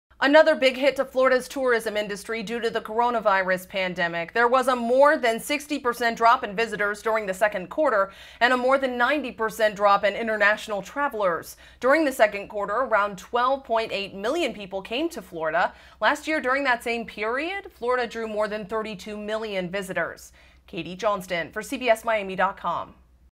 Florida Tourism Numbers Plummet In Second Quarter
Florida’s vital tourism industry suffered an estimated 60.5 percent drop in visitors as the COVID-19 pandemic hit hard during the year’s second quarter, with international travel off more than 90 percent. Katie Johnston reports.